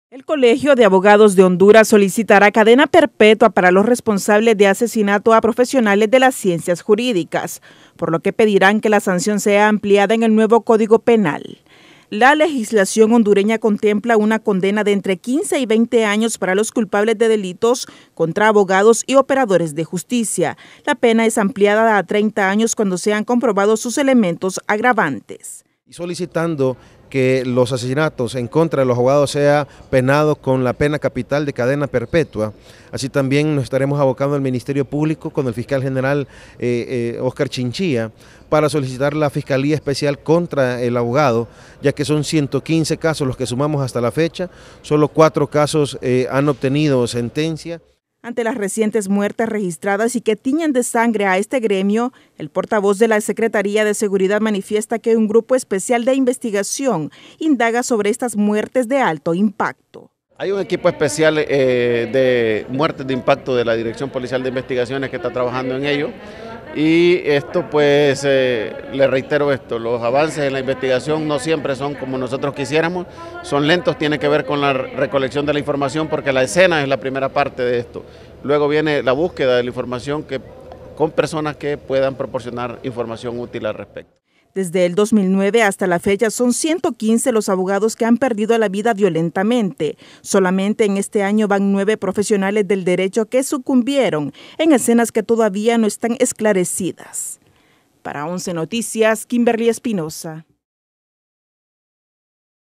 Colegio de Abogados de Honduras pide cadena perpetua para asesinos de togados
El Colegio de Abogados de Honduras, solicitará cadena perpetua para los responsables de asesinato a profesionales de las Ciencias Jurídicas, por lo que pedirán que la sanción sea ampliada en el nuevo Código Penal.

La legislación hondureña contempla una condena de entre 15 y 20 años para los culpables de delitos contra abogados y operadores de justicia. La pena es ampliada a 30 años cuando sean comprobados sus elementos agravantes.

Ante las recientes muertes registradas y que tiñe de sangre a este gremio,  el portavoz de la Secretaria de Seguridad manifiesta que un grupo especial de investigación indaga sobre estas muertes de alto impacto.

Desde 2009 a la fecha, son 115 los abogados que han perdido la vida violentamente. Solamente este año van nueve profesionales del derecho que sucumbieron en escenas que todavía no están esclarecidas, destacó.